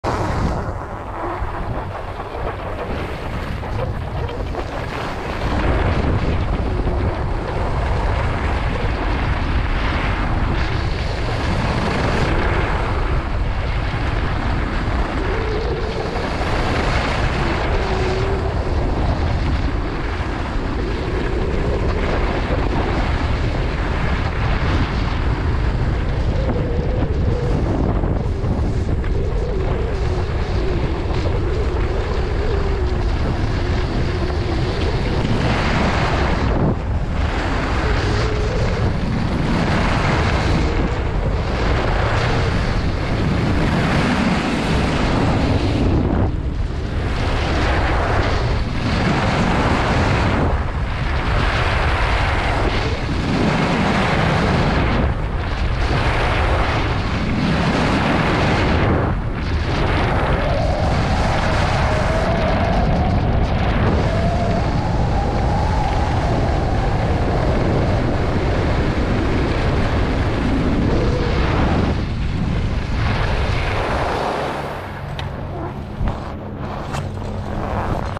Skiing Anniversary via Cindy Top at Cascade Mountain
Anniversary is a very fun black diamond at Cascade Mountain. It is best to go fast on this trail due to its narrow width. The steepness is similar to Cindy Pop and North Wall. To get to the run, stay to the right side of North Wall until you get to the top, which is just before the steep section of North Wall.